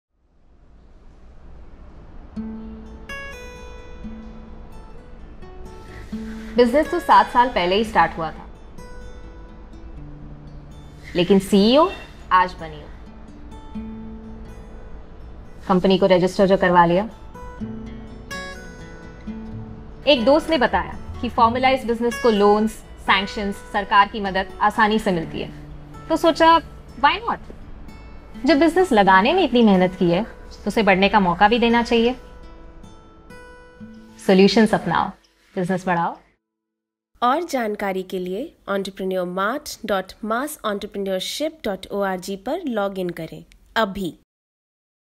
How Entrepreneur Mart helped a young businesswoman formalize her enterprise
Growth takes time and perseverance. But what can one do to ensure and accelerate their growth in the right direction? For Meena Bisth, that step was to formalize her small business. With a registered name, she could enjoy the various benefits that were on offer.

When it comes to business solutions, Global Alliance for Mass Entrepreneurship (GAME) and its partners have got you covered. The Entrepreneur Mart on GAME’s website boasts of several resources which can help an entrepreneur tide over any kind of issues they face - be it legal, financial, or sales! No waiting line, and no paperwork of any sort. Just log on to (add link) and connect with some of the best consultants in their fields.